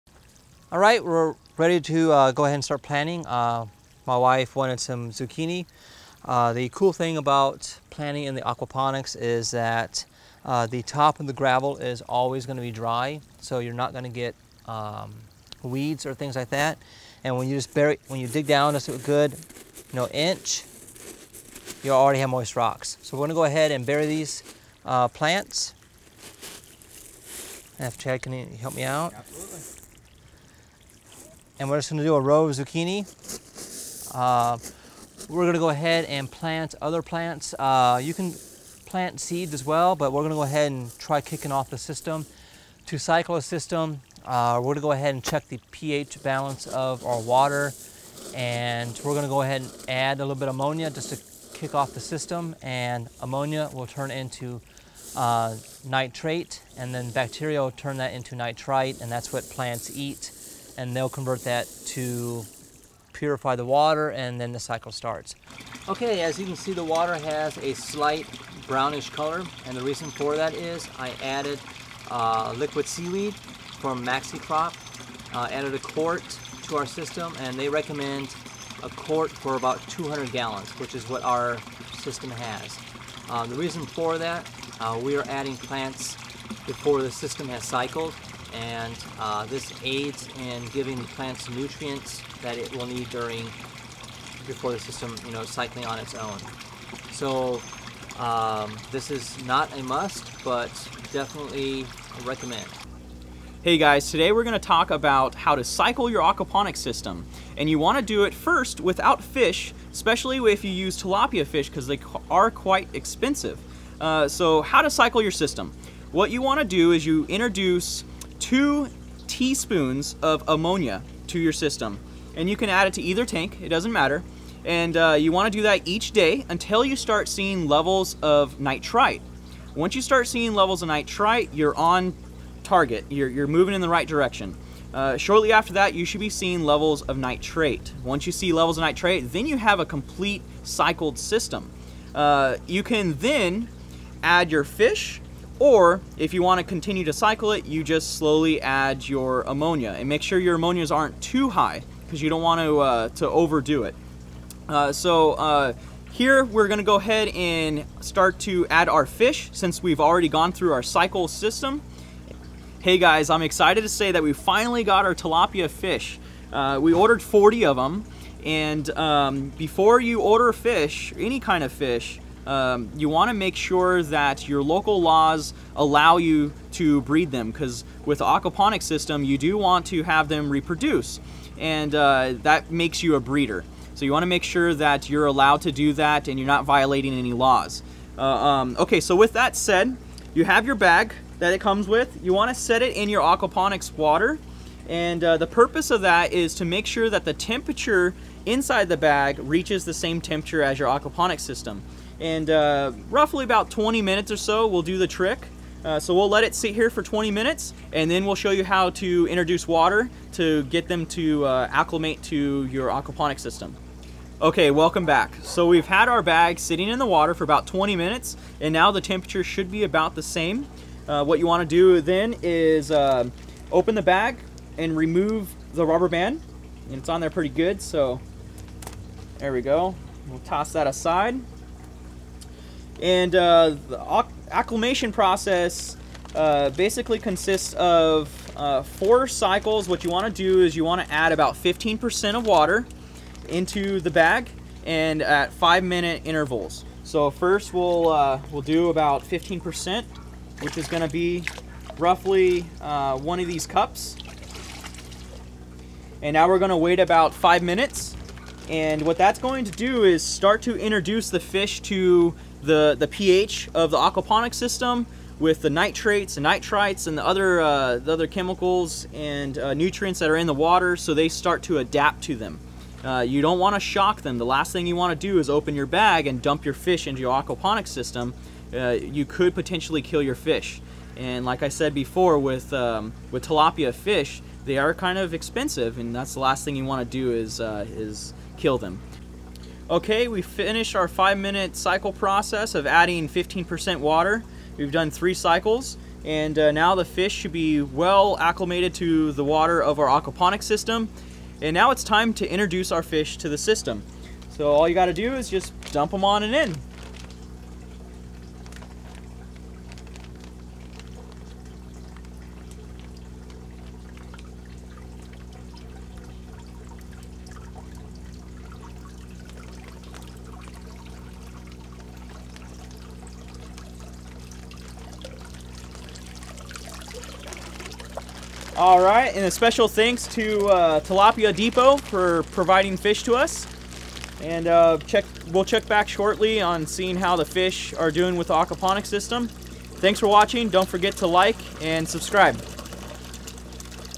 Aquaponics BackYard System - Introduce Fish Part3
In this video we introduce our fish. We chose to use Tilapia which are more tolerant of water temperature changes. Please make sure to check your local laws and regulations as they do vary from state to state.
We purchased our fish from Tilapiaworld and were very please with the fast service and answered all of our questions.